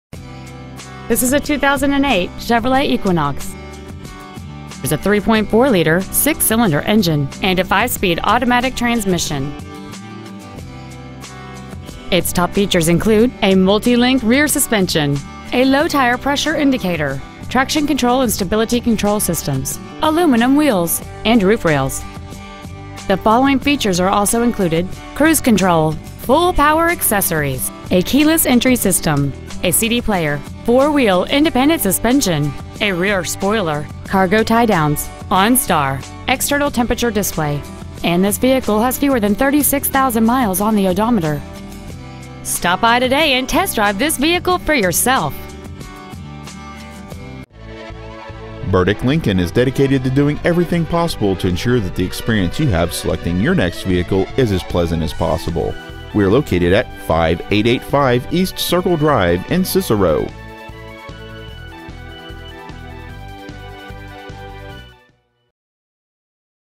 2008 CHEVROLET EQUINOX Cicero NY
Year : 2008
 Make : CHEVROLET
 Model : EQUINOX
 Engine : 3.4L V6
 Trans . : AUTO 5SPD
 Exterior : SL
 Miles : 35,947

 Stock : B089R9439